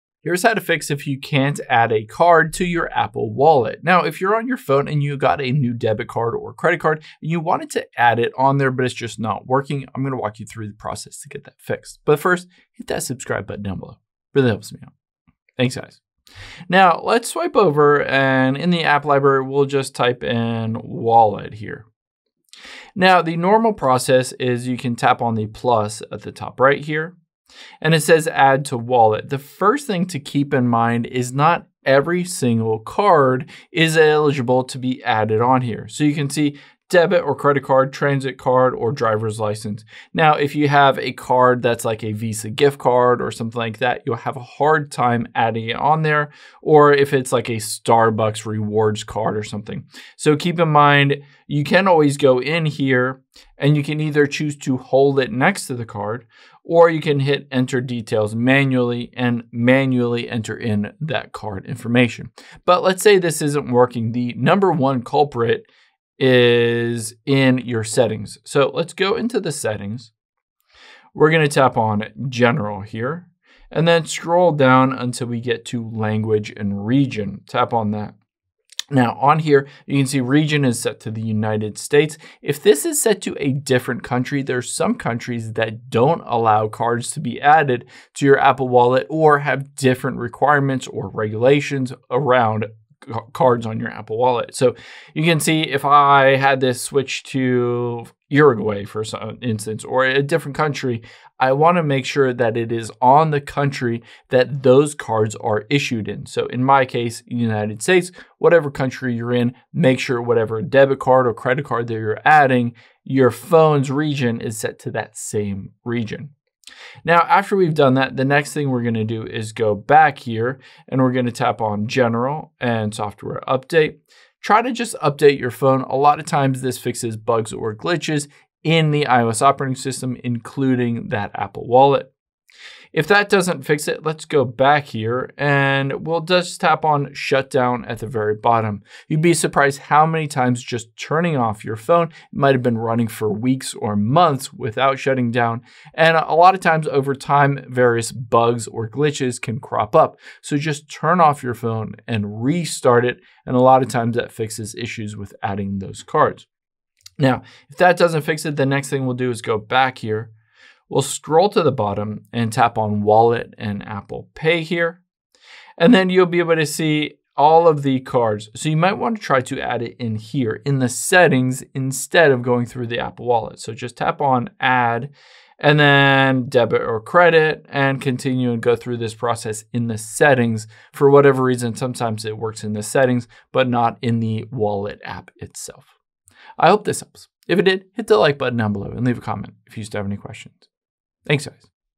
How To Fix Can't Add Card To Apple Wallet
Here's how to fix your iPhone Apple Wallet if you cant add a card, whether it's a debit or credit card, gift card or anything else.